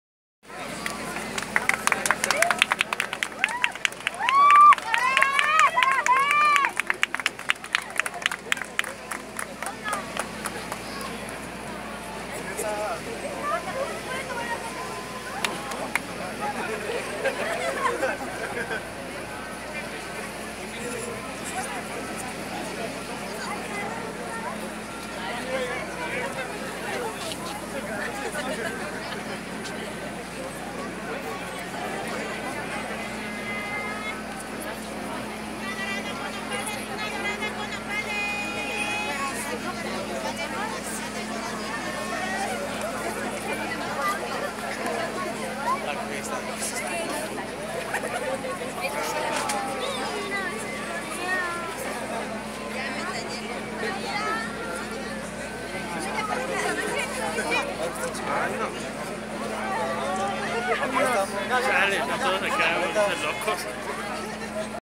818 World Archery Championships. Mexican people love korean athletes
World Archery Championships, Mexico City, October 21, 2017